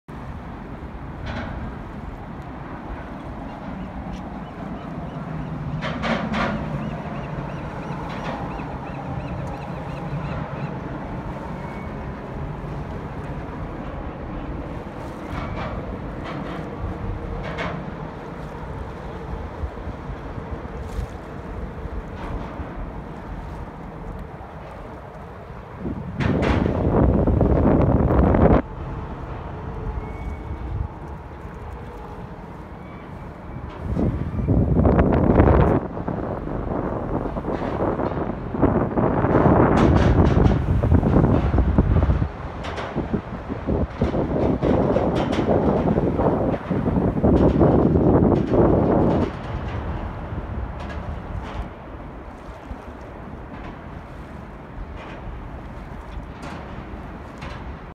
South Bay Sailing - Catalina 27 - 201112
Heading north of the San Mateo Bridge - made it up to Coyote Point today, then had a nice run back to Spinnaker.